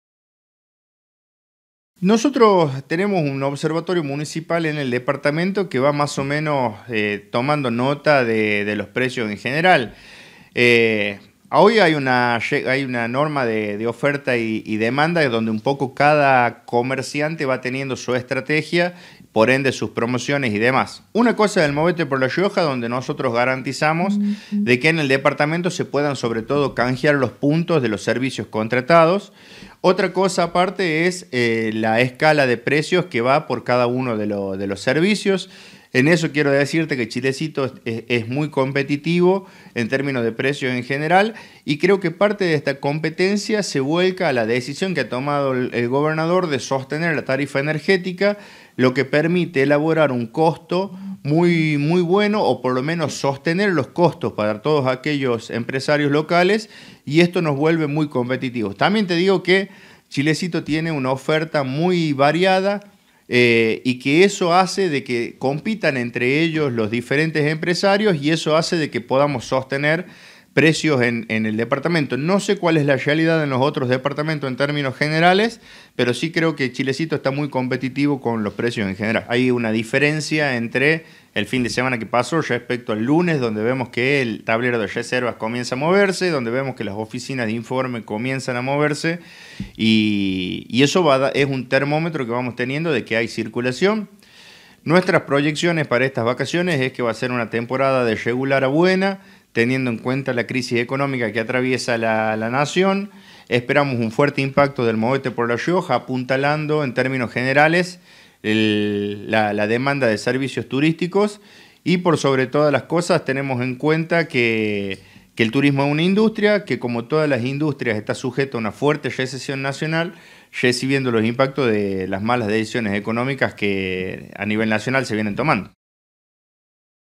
DiarioChilecito.com
Turismo municipal destacó la competitividad de Chilecito en términos de precios, y comentó que a pesar del impacto positivo del programa "Movete por La Rioja", "el turismo es afectado por las medidas de Nación"